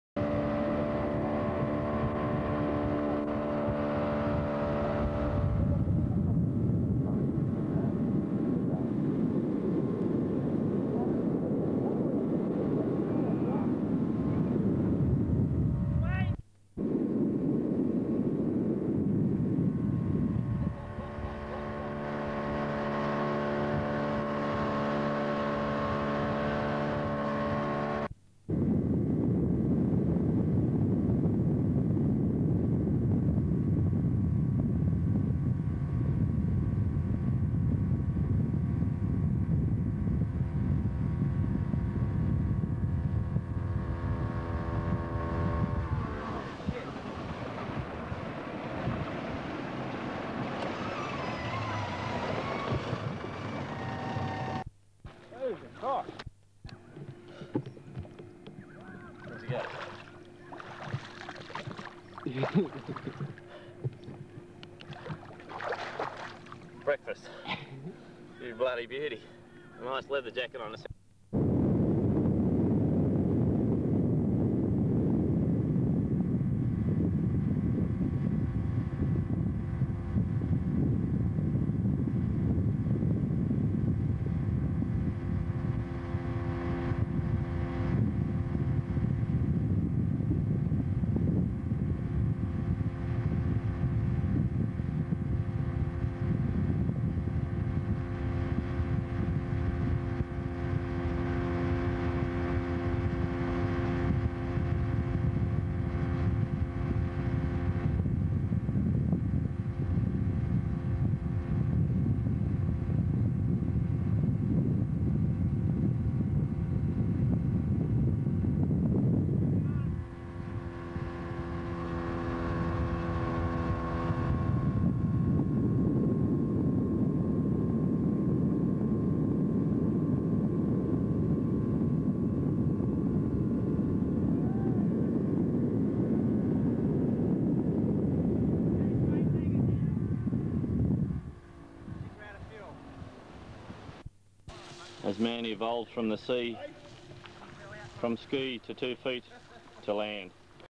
Barefooting Wayne Rogers 1985
Barefooting South West Arm. You will note that in first part of the video things were going great until the driver of the boat ran aground when he hit a sandbar which left me standing in knee deep water where I happened to look down and see a deep bodied leather jacket swimming around me which is why I picked it up and said "Yum, breakfast". I did let go. In the second part of the video I did my personal best barefooting for over a minute including my sideways barefoot whip until my feet started burning and I had to let go of the bar.
And no, it isn't Russell Crowe as many people have joked about, its definitely me  : - )